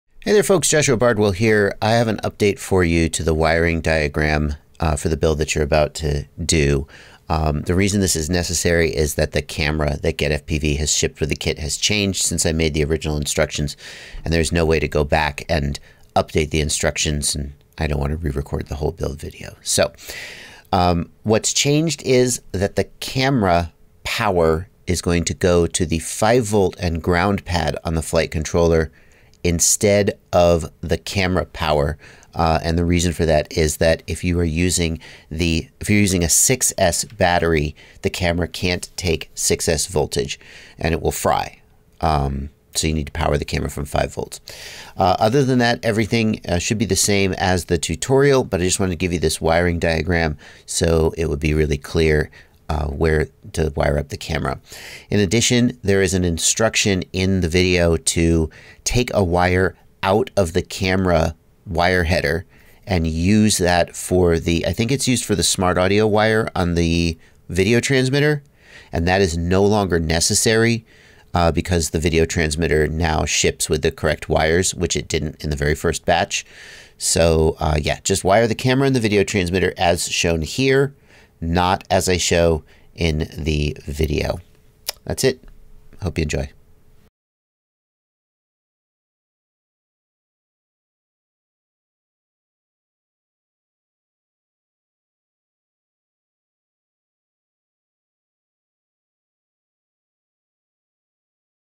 FPV Drone Xilo Budget Build Updated Wiring Instructions IMPORTANT
The wiring instructions have changed since my original tutorial. Watch this please! Don't fry your camera!